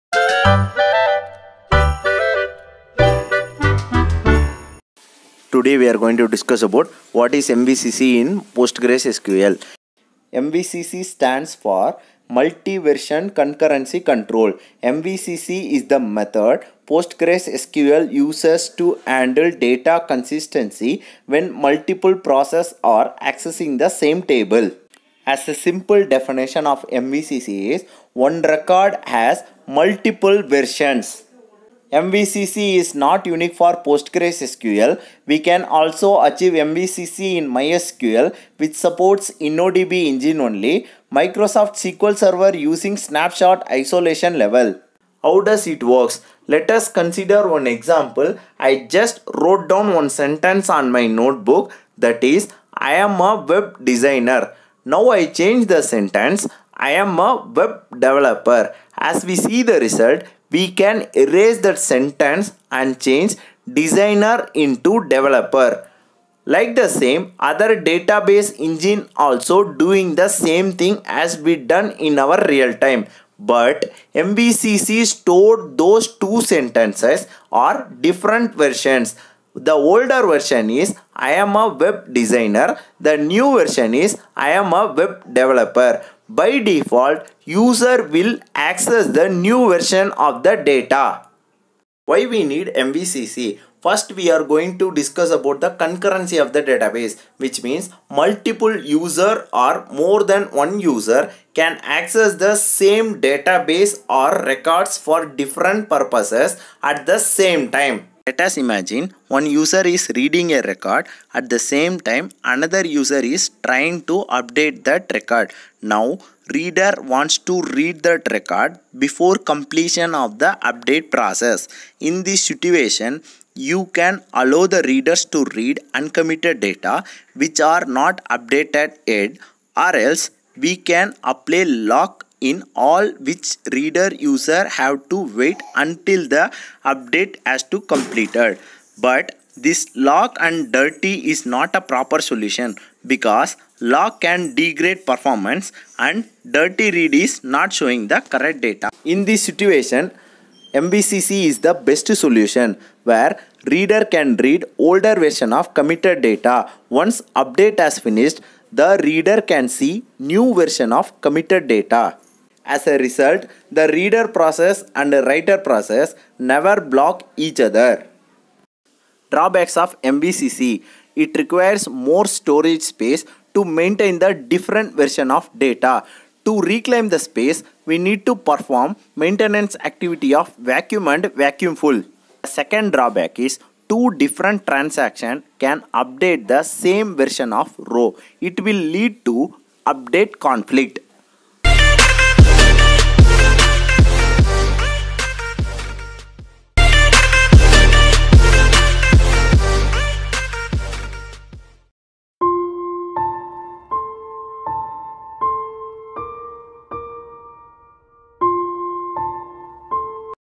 Tutorial 01-What is MVCC in PostgreSQL
In this session we are going to discuss about what is mvcc in PostgreSQL?

i) MVCC stands for Multi-Version concurrency control.

ii) MVCC is the method PostgreSQL uses to handle data consistency when Multiple processes are accessing the same table.

iii) As a simple definition is one record has multiple versions

iv)  MVCC is not unique for PostgreSQL,  we can also achieve MVCC in Mysql (Innodb engine only), Microsoft SQL server using “ Snapshot Isolation Level”.

Why we need MVCC?

I) It will allow multiple users or more than one can access the same  records at the same time

ii) MVCC provides concurrent access to the database.

iii) where readers can see the old committed version of data and once the update finished, the reader can see a newly committed version of the data. The read process and write process never block to each other.

Drawbacks:

- It requires more storage space to store different versions of data. We can reclaim that through maintenance activities of vacuum and vacuum full.

-  In the MVCC, the same data causes an update conflict because two different transactions can update the same version of the row.